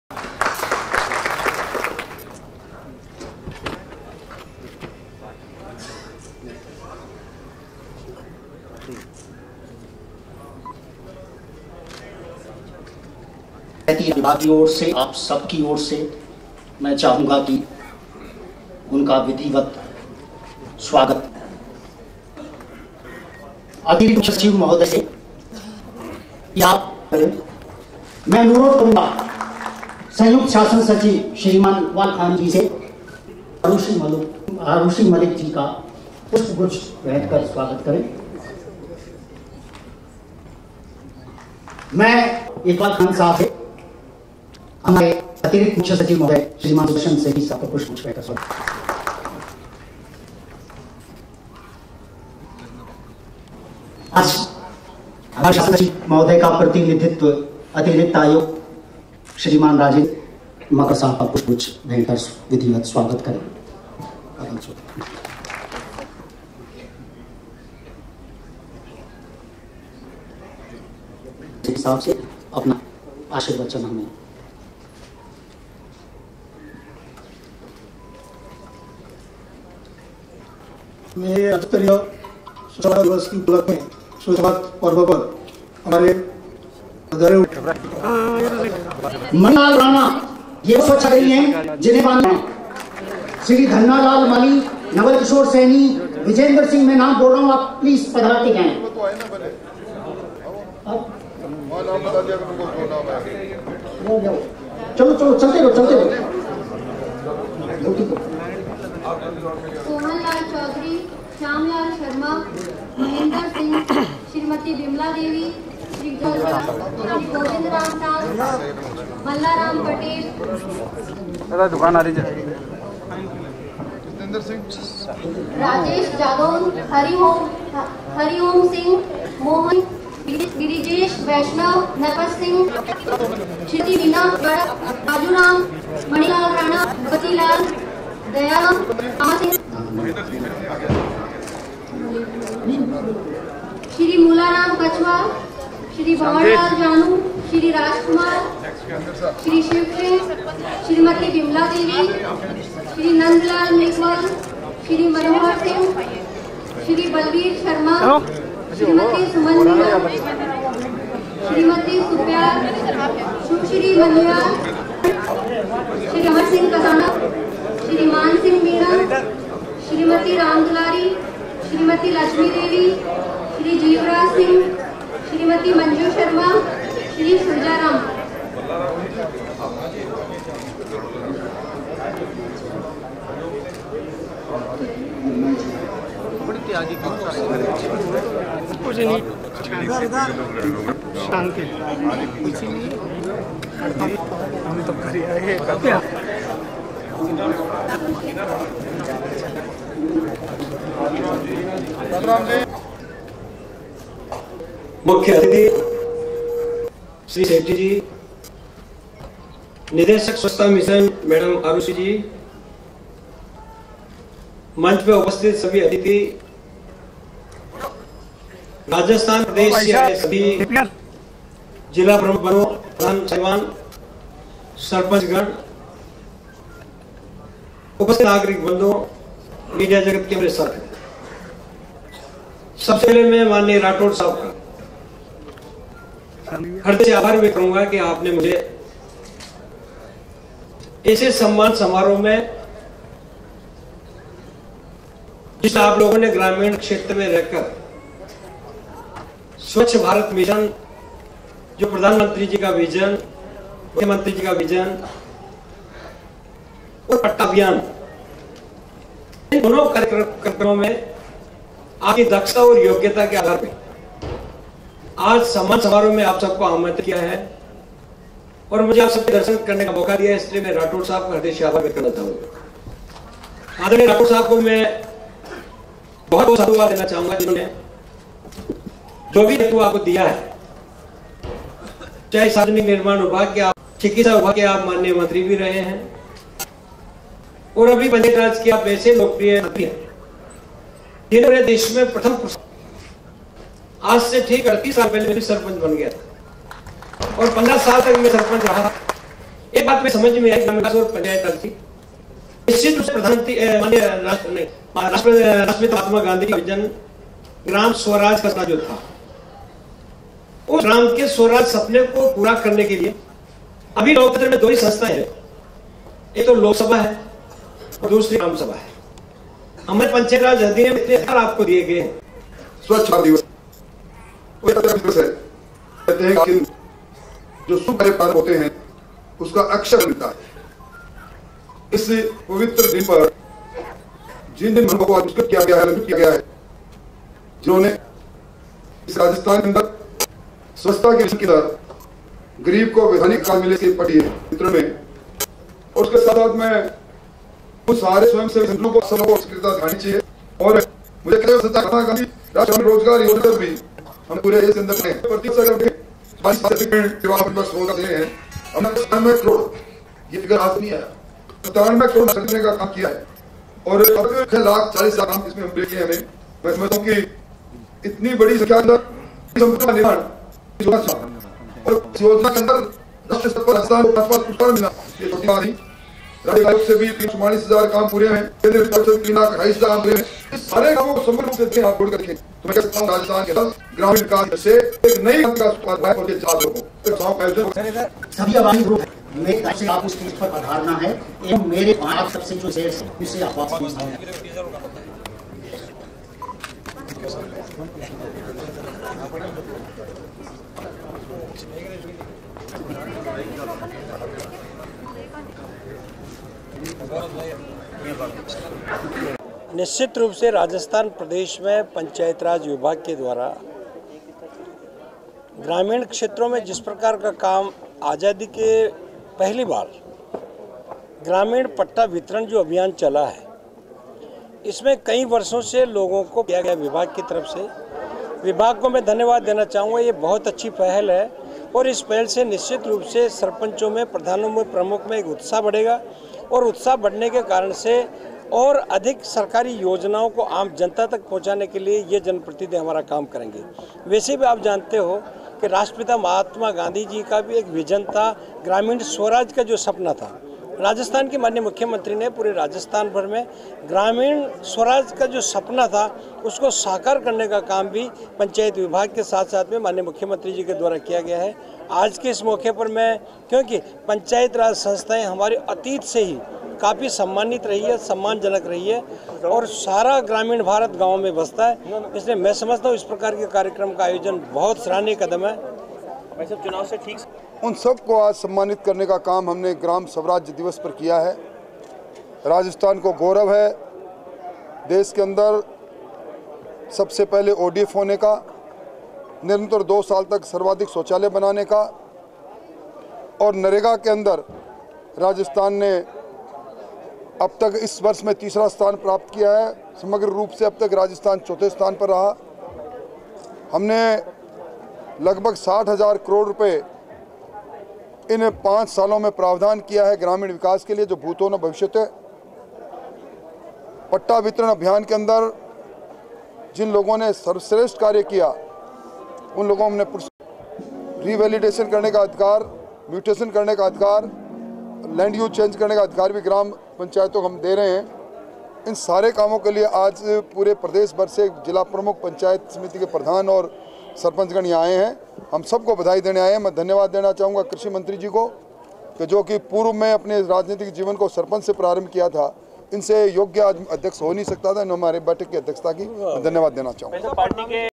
स्वच्छ भारत अभियान के तहत राज्यस्तरीय उत्कृष्टता पुरस्कार समारोह का आयोजन
राजस्थान ग्रामीण विकास एवं पंचायती राज विभाग की ओर से ग्राम स्वराज अभियान और स्वच्छ भारत अभियान के तहत राज्यस्तरीय उत्कृष्टता पुरस्कार समारोह का आयोजन किया गया... समारोह में स्वच्छ भारत अभियान के तहत पहले ओडीएफ होने वाली पंचायती राज संस्थाओं और पंडित दीनदयाल उपाध्याय पट्टा वितरण अभियान में सर्वश्रेष्ठ काम करने वाली 123 पंचायती राज संस्थाओ को सम्मानित किया गया. इस मौके पर ग्रामीण विकास एवं पंचायती राज मंत्री राजेन्द्र राठौड़ और कृषि मंत्री प्रभुलाल सैनी भी मौजूद रहे. समारोह के दौरान राठौड़ ने कहा कि राज्य को सबसे पहले ओडीएफ होने का गर्व है. सरकार ने पिछले 4 सालों में ग्रामीण विकास के लिए लगभग 60 हजार करोड़ रूपए का प्रावधान किया है. जो अभी तक किसी ने नहीं किया...